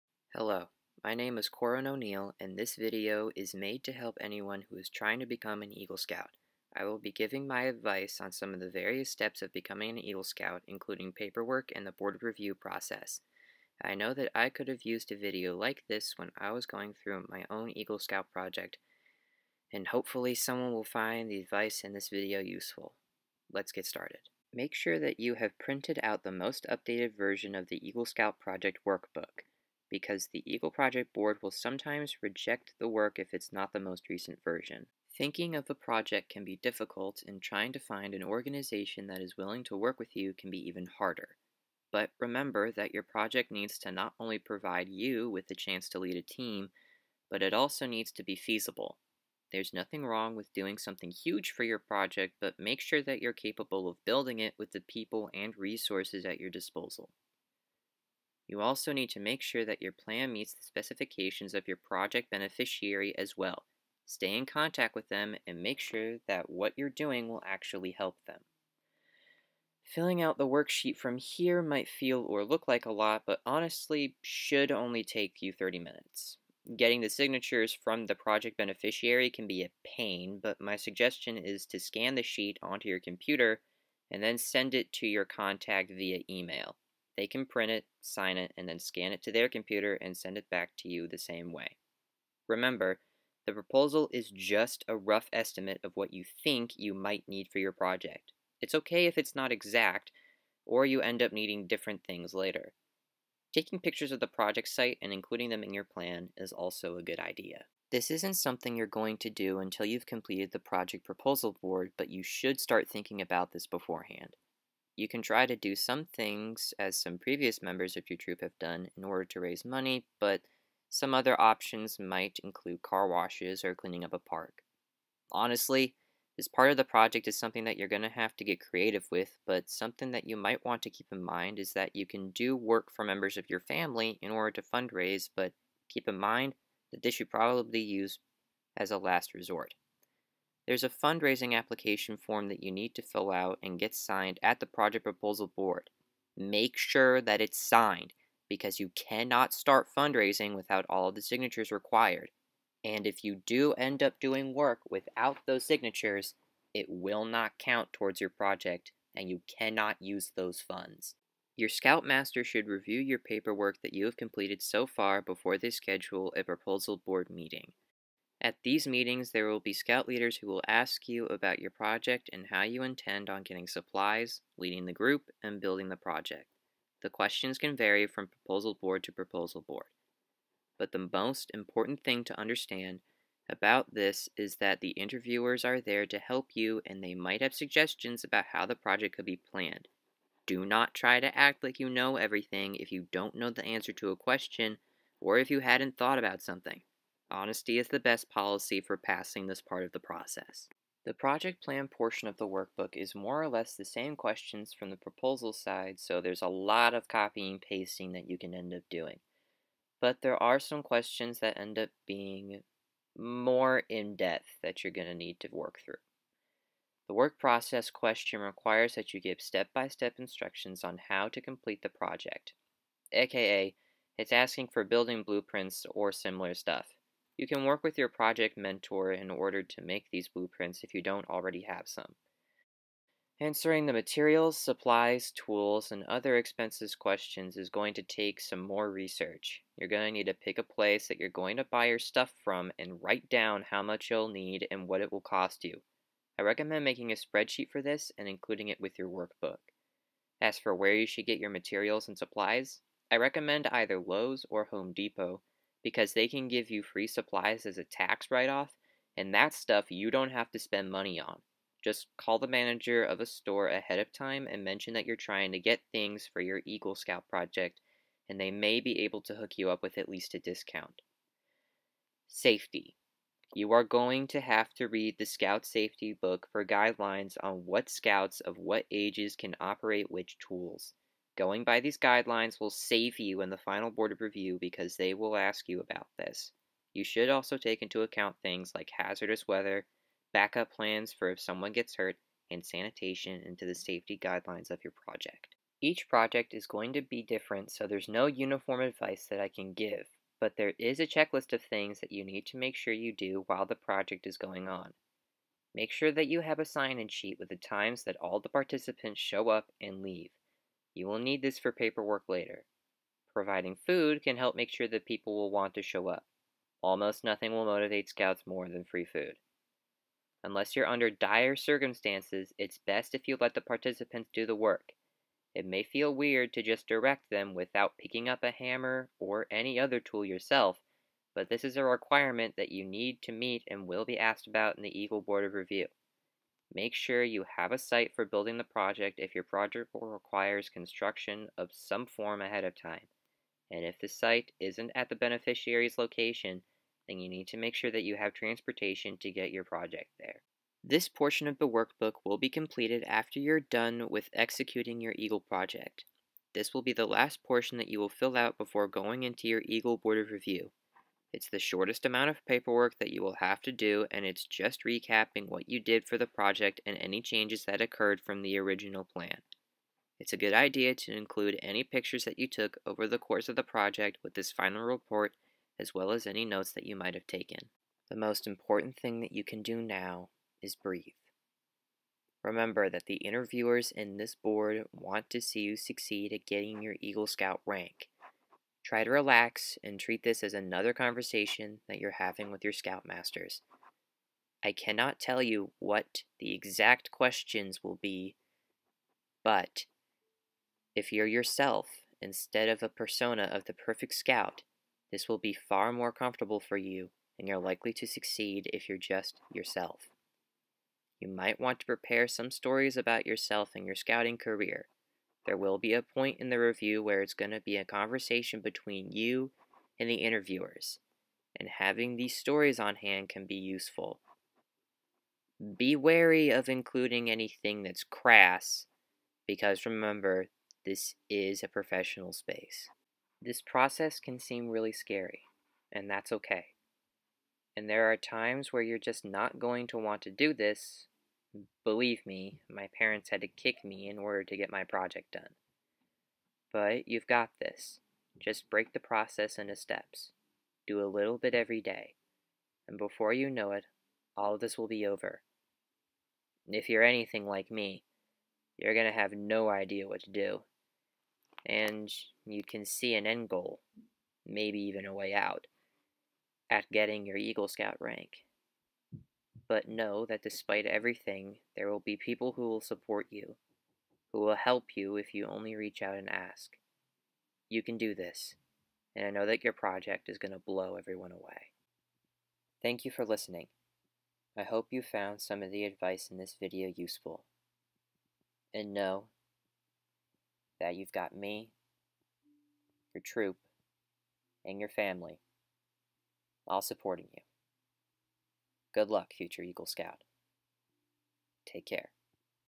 Eagle Scout Project Advice
A brief advice video going through the major steps of getting your eagle rank and what to keep in mind.